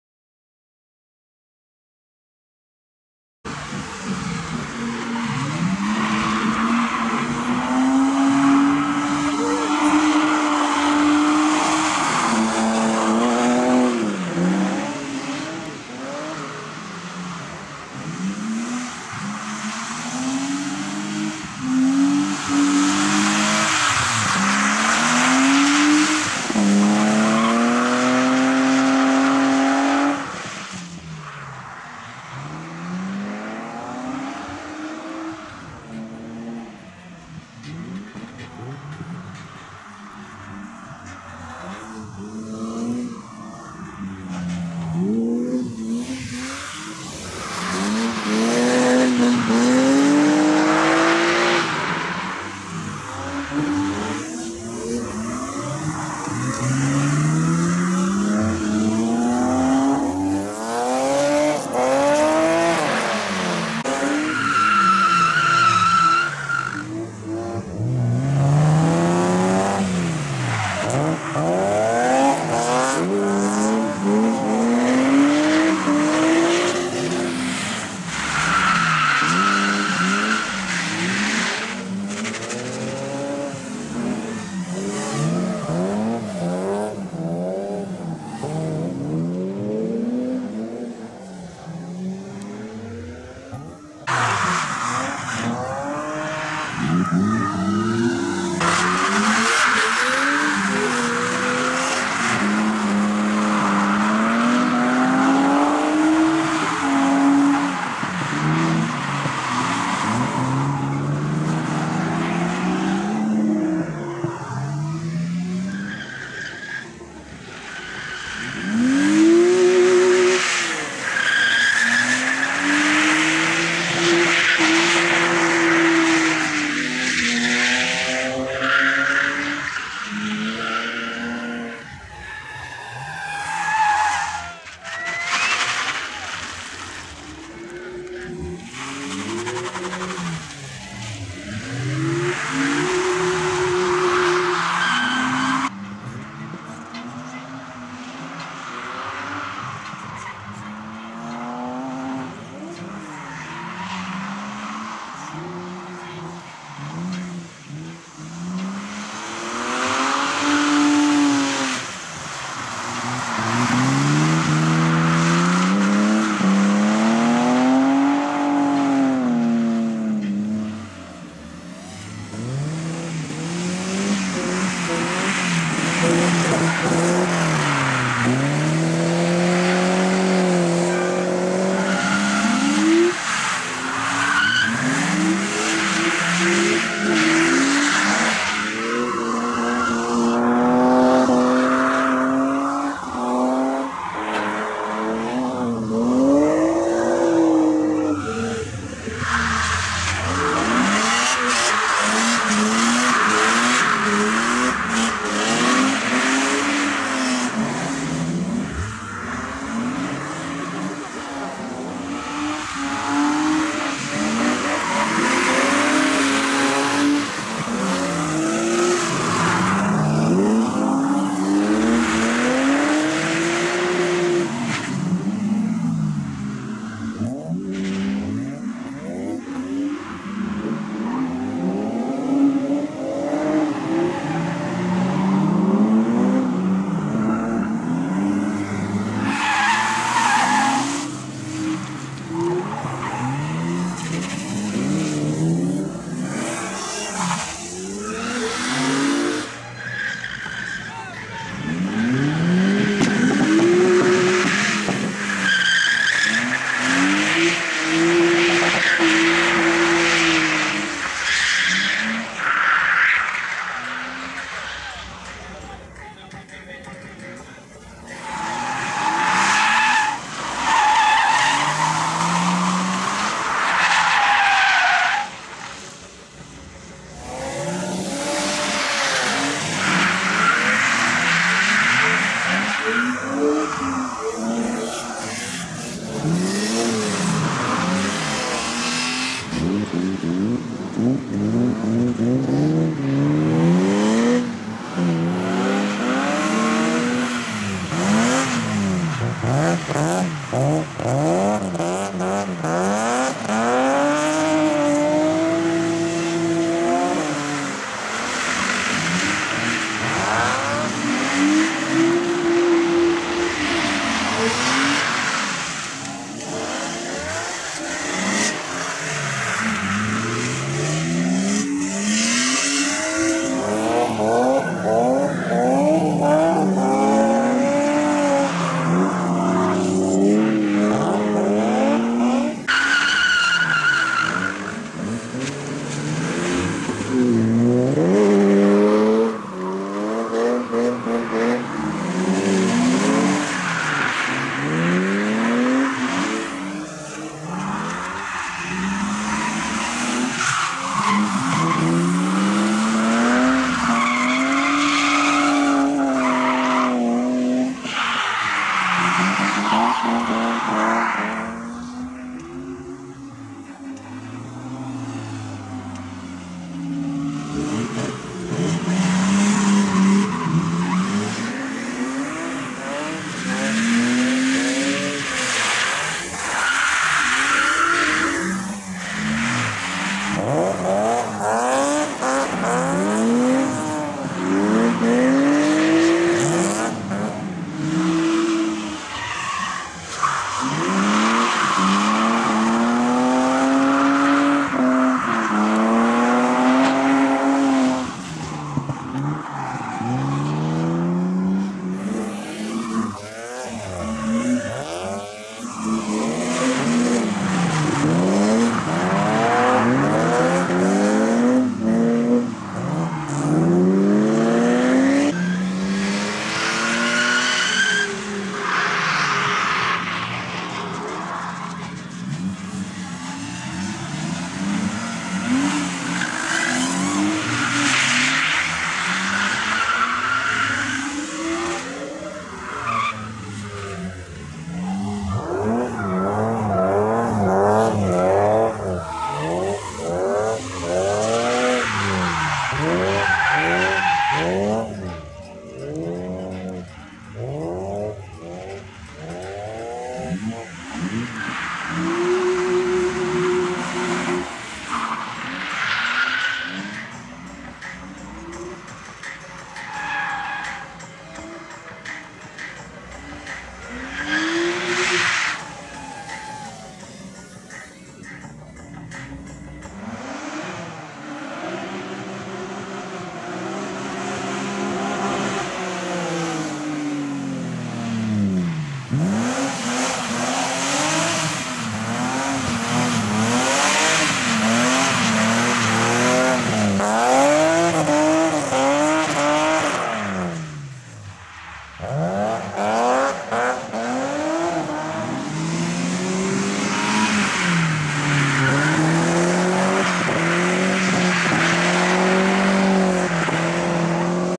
Drift Show Rass'Auverdrive - 1st Edition - Sep 2022 - Rallye Vision
Voici un petit résumé du Drift show sur la piste de karting du rassemblement de Marcillat en Combraille 1ère édition situé en Auvergne.
Un grand merci a l'asso Auverdrive pour cette superbe journée malgré le temps pourris

N'hésitez pas a partager la vidéo, l'aimer et la partager si vous le souhaitez !